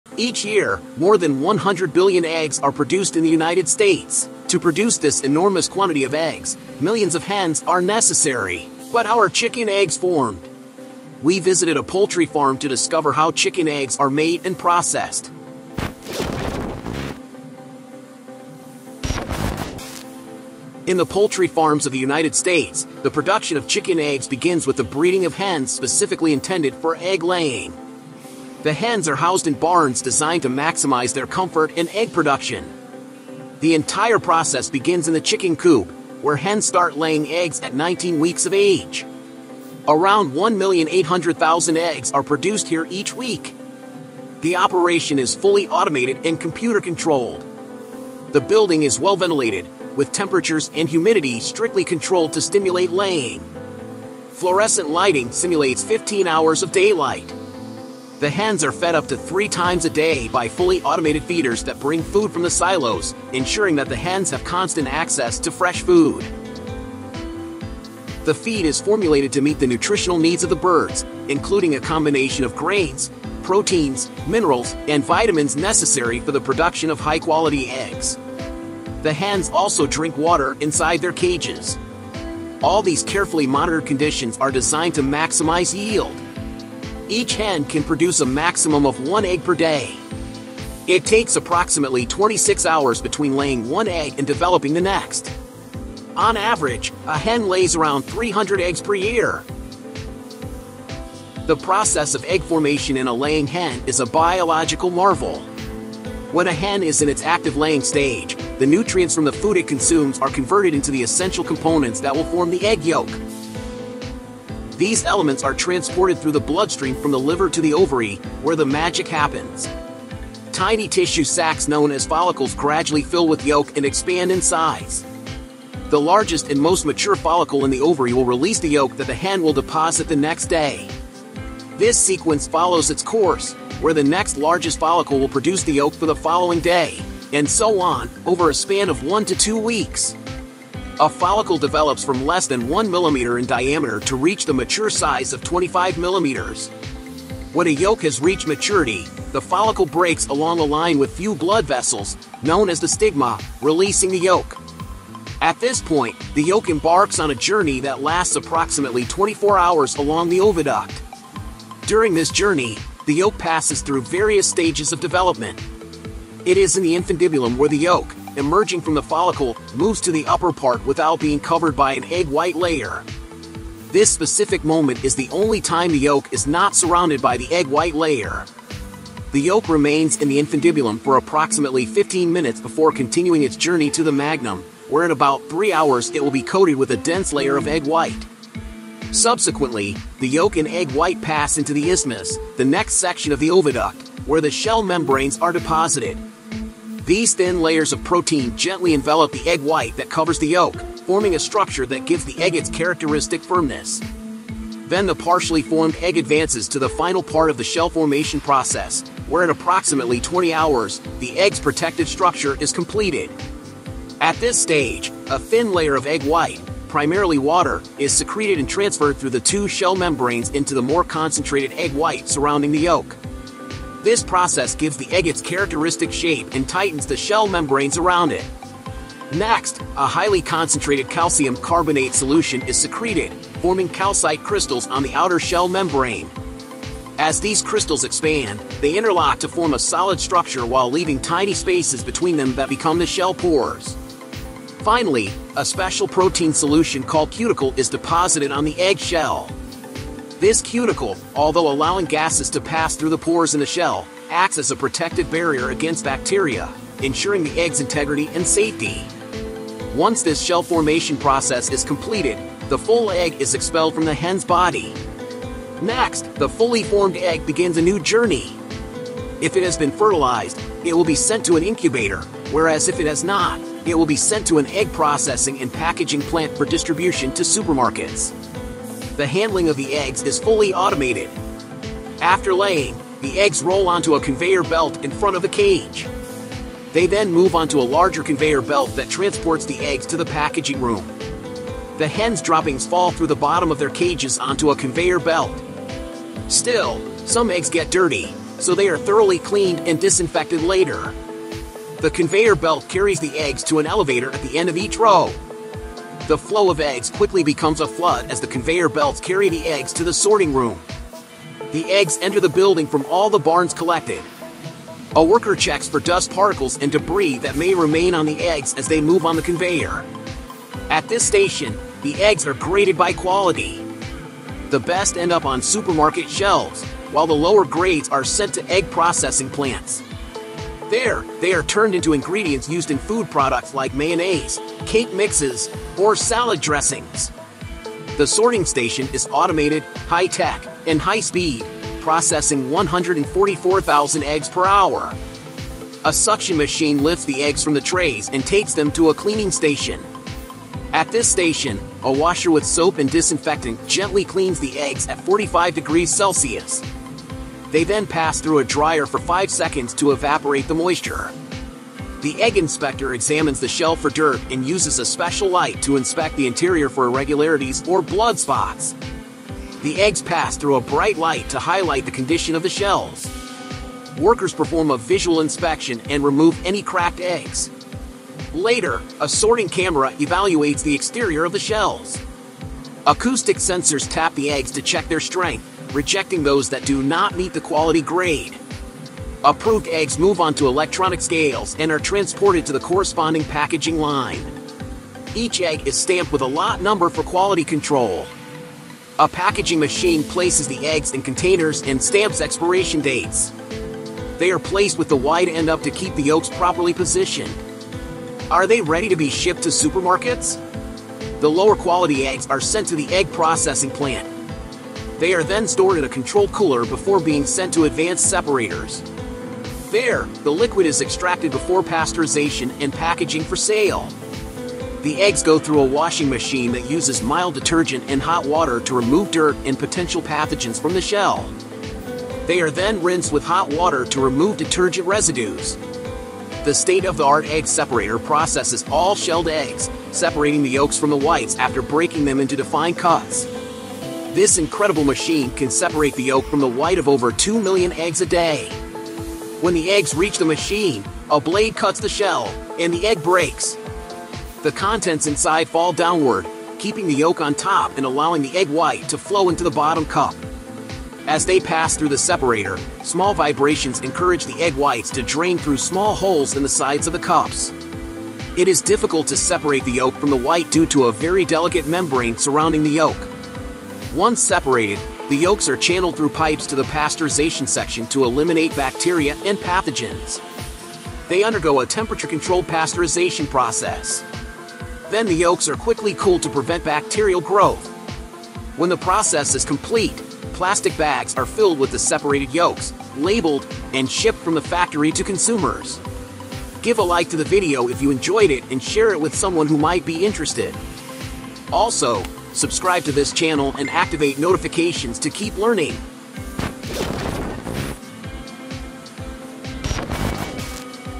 HOW 100 BILLION 🥚 EGGS Are Produced Annually in the United States
In this video, we explore the fascinating process behind the production of chicken eggs in the United States, where over 100 billion eggs are produced annually. From poultry farms to the egg formation process inside the hen, we'll take you step by step through the entire cycle. You'll discover how hens are raised in specialized facilities, how the eggs are formed, processed, and packaged, and the advanced technology that ensures their quality and safety. Whether you're curious about how eggs are made or interested in modern farming practices, this video offers a comprehensive look at one of the most common foods in the world.